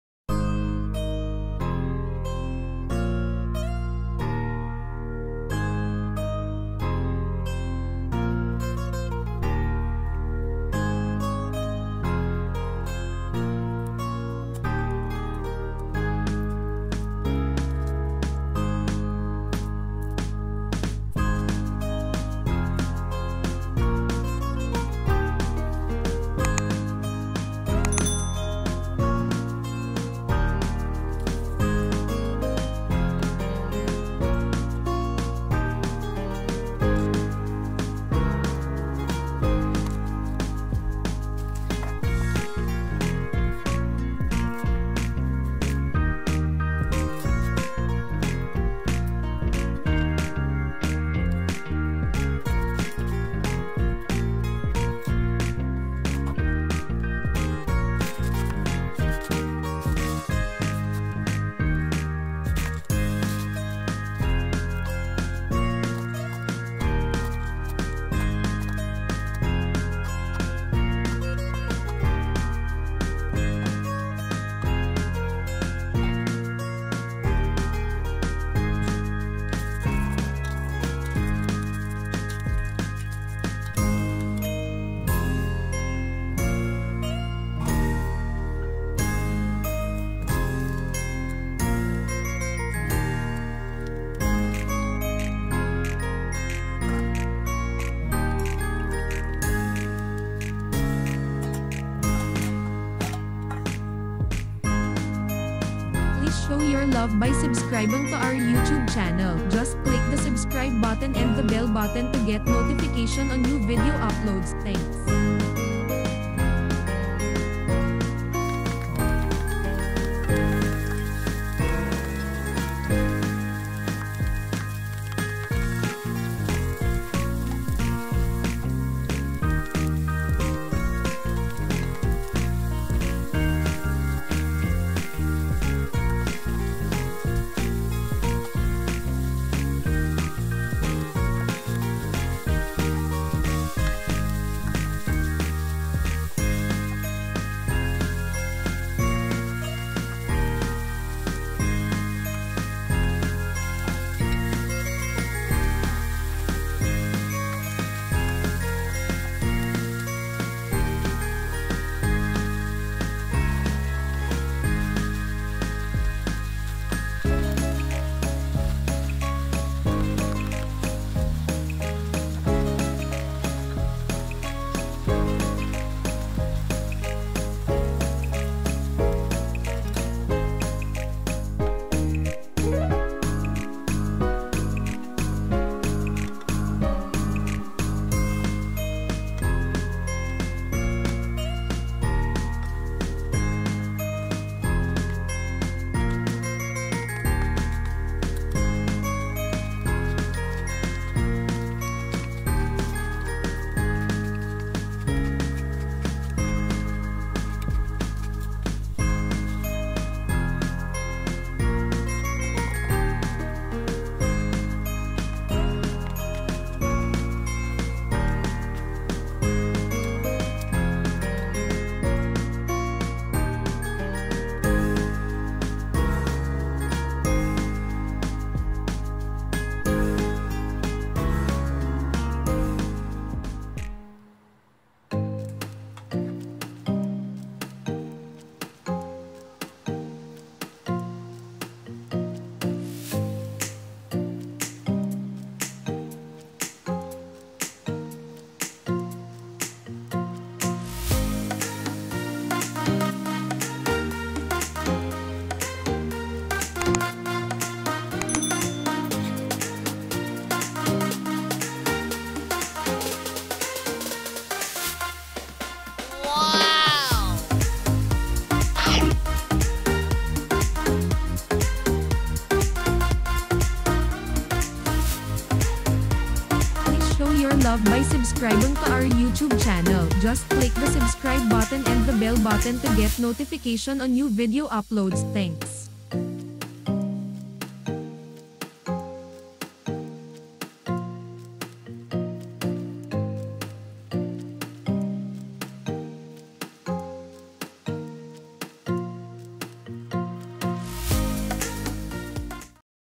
GAWIN ITO SA REPOLYO NYO AT MAPAPA OH MAY GULAY! KA SA SARAP! Yummylicious!
Ingredients:
1 large cabbage, slice into large strips
1/4 kilo shrimps with shell
1 Tbsp. oyster sauce
1 carrot, slice into strips
3 cloves garlic, minced
1 pc onion, chopped
1 cup water
1 Tbsp. cooking oil
1/4 tsp. ground black pepper
1 tsp. salt or patis